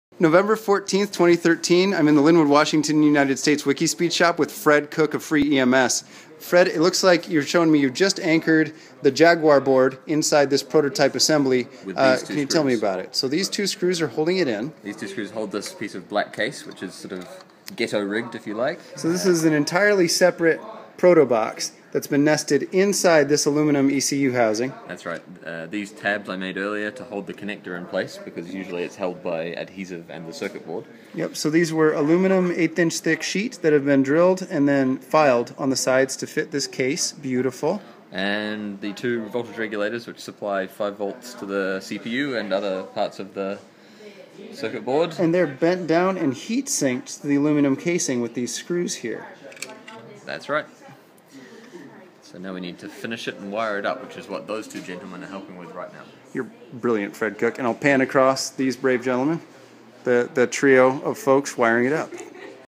WIKISPEED FreeEMS Jaguar Enclosure
Fred Cooke of FreeEMS (engine management system) shows us how the FreeEMS Jaguar board is held securely in a prototyping enclosure which is then held in an aluminum protective ECU shell.